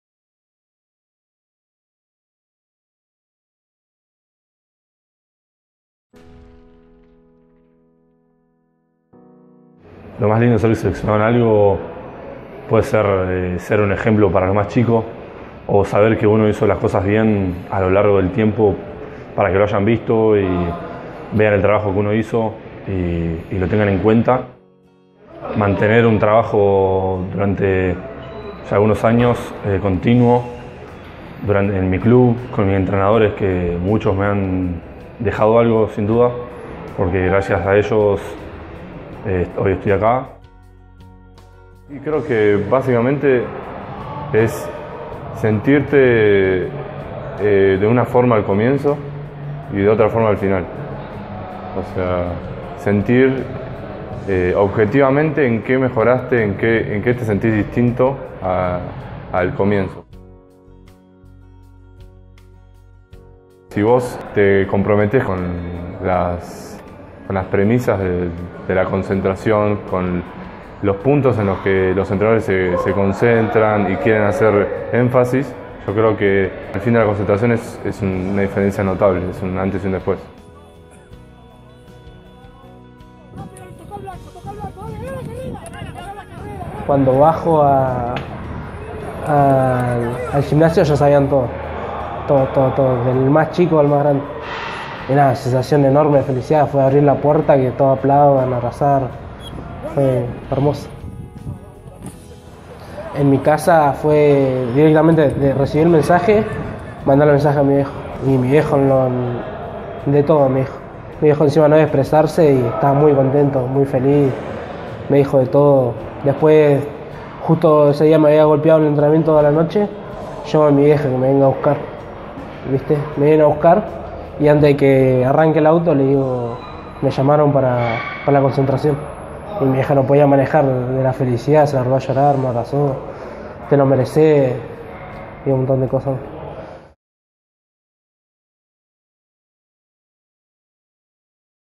Dieciséis voces, dieciséis uniones. (Parte 1)
Los juveniles Tomás Ferreyra (Entrerriana), Agustín Toth (URBA), y Ezequiel Uvieda (Austral) cuentan su experiencia.